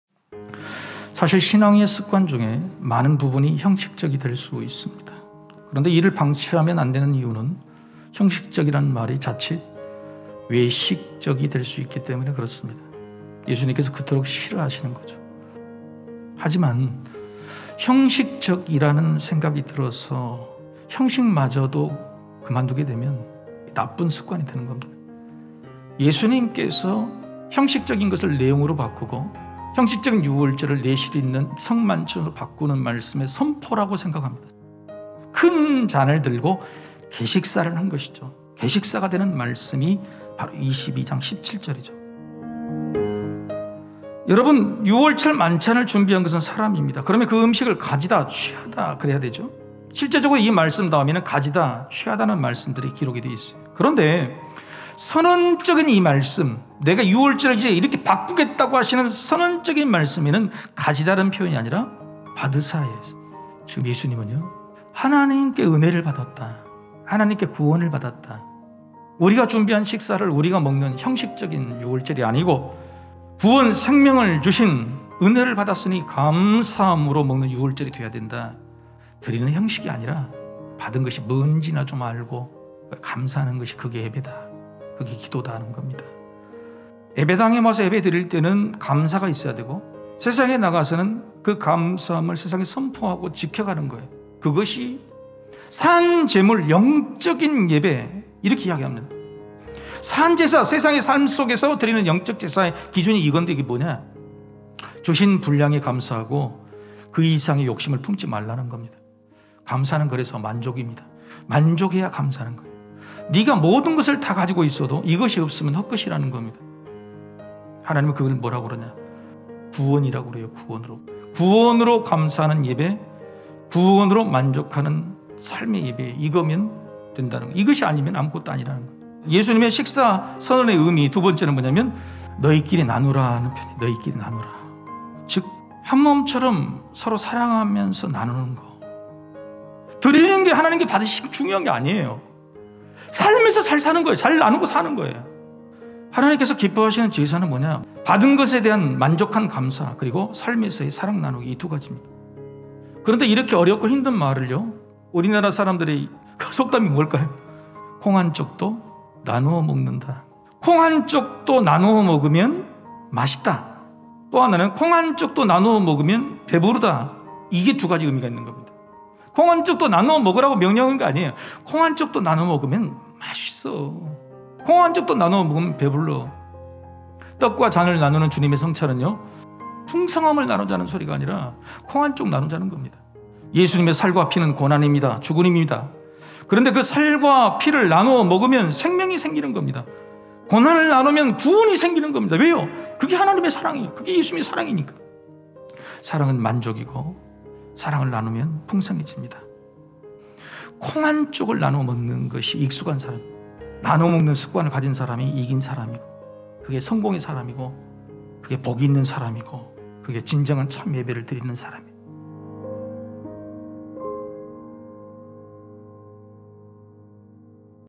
3분설교| 나눠 먹는 이기는 습관 | 강주성목사
———한번 더 묵상하는 3분 설교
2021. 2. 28. 주일 설교 "나눠 먹는 이기는 습관"중에서
성경 본문 | 시편 50 : 1 - 23

주일예배 설교방송 안내
CBS라디오(FM 103.1) 매 주일 오전 09:05